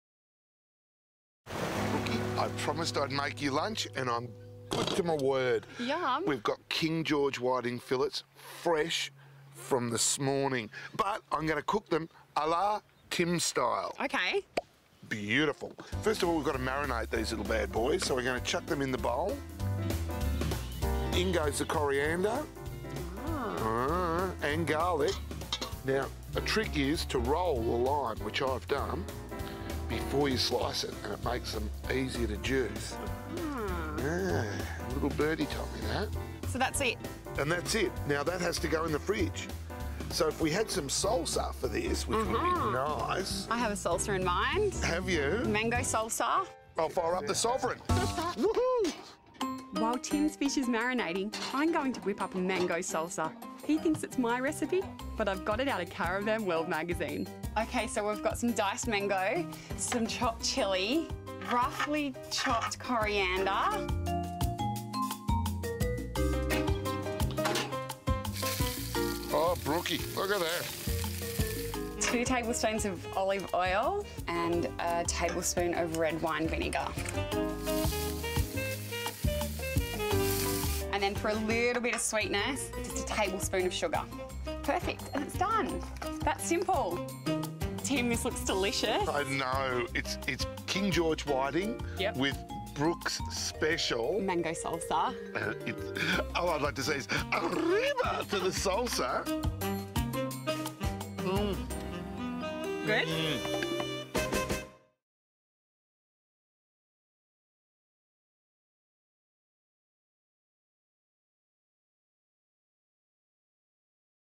Tim & Brooke from 'Discover Downunder' cook using the Sovereign BBQ No.2 | Dometic Group
Tim Smith and Brooke Hanson from 'Discover Downunder' cook up some 'King George Whiting' on the Sovereign BBQ distributed by Dometic Group. Brooke also whips up a mango salsa to compliment the whiting.